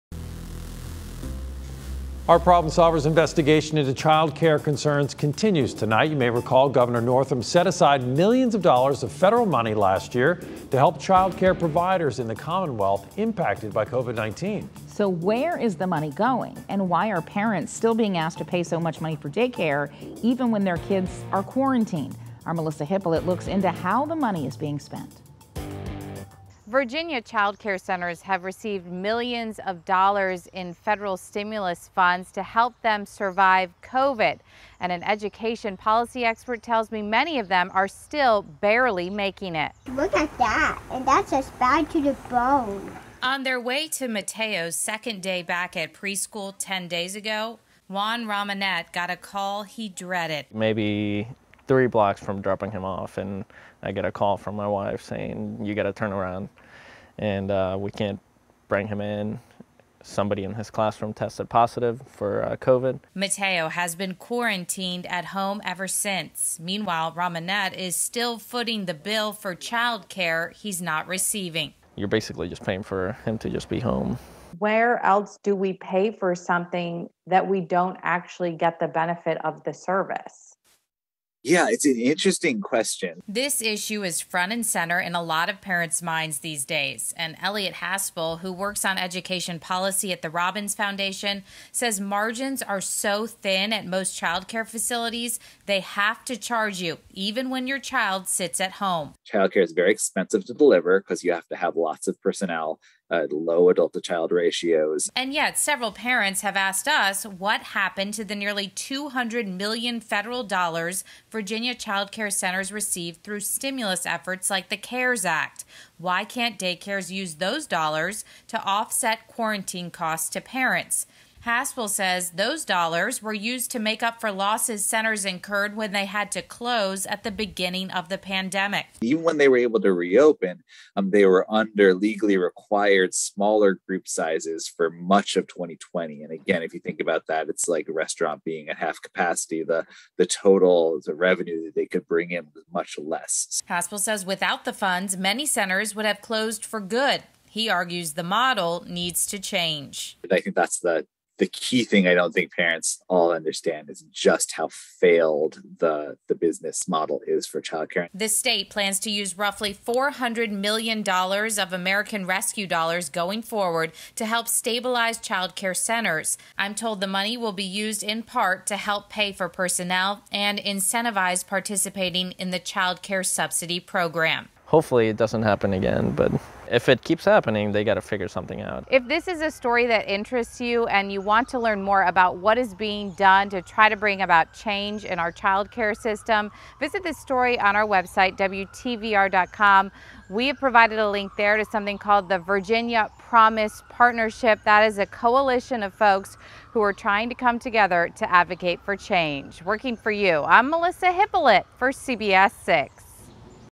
Parents stuck paying for day care when kids are quarantined
Parents are frustrated with COVID-19 keeping kids out of daycare and still paying for the costs. They're asking, where is the federal money?